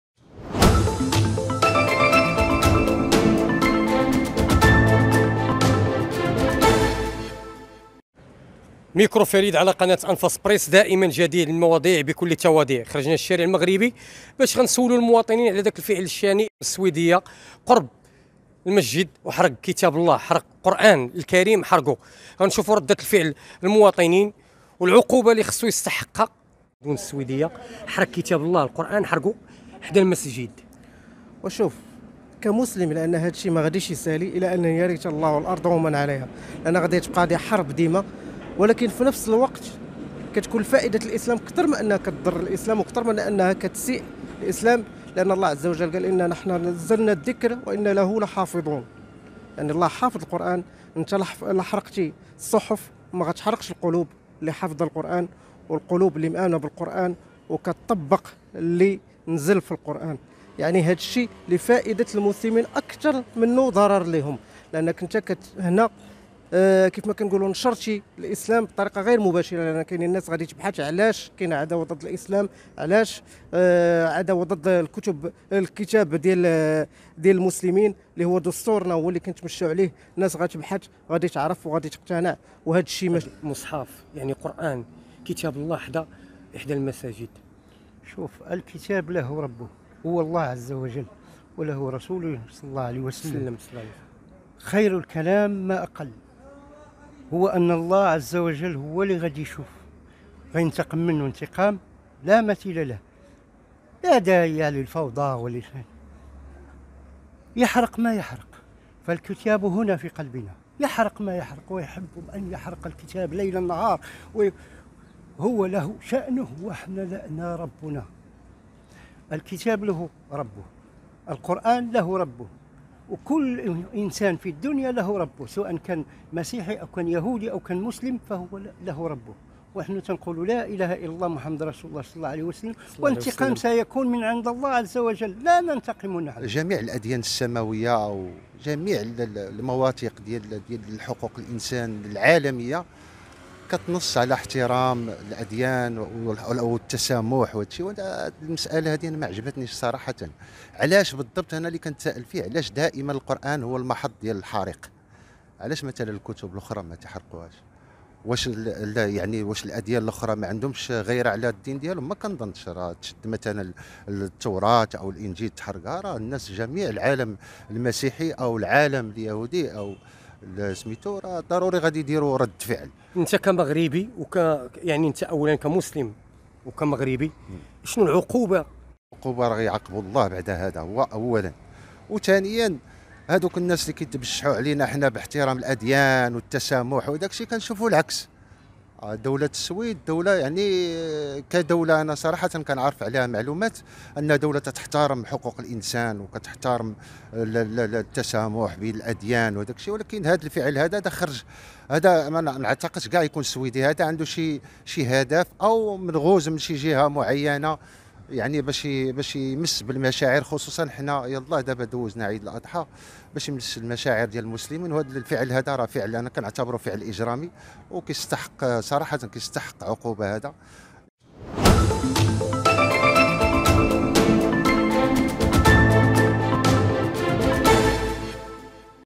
مواطنون يقترحون هذه العقوبة لحارق كتاب الله في السويد
يتصاعد الاستنكار جراء حرق احد العراقيين بالسويد المصحف الشريف، هنا في المغرب، اعتبر عدد من المواطنين هذا العمل بالشنيع، والذي يحض على الكراهية، ويقترح هذا العقاب..